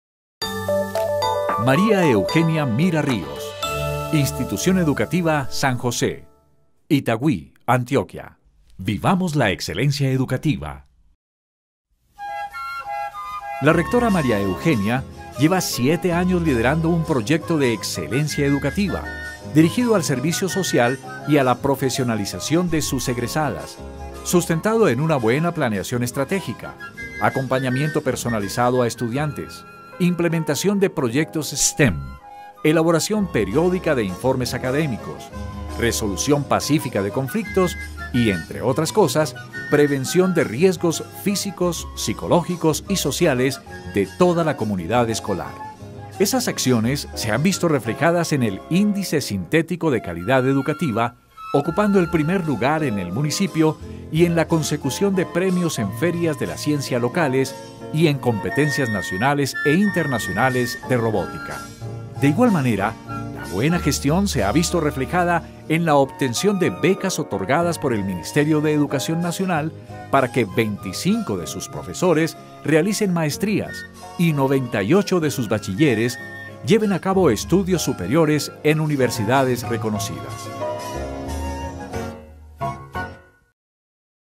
Rectora María Eugenia Mira, de Itagüí, Antioquia
María Eugenia Mira Ríos, rectora de la Institución Educativa San José, en Itagüí, Antioquia.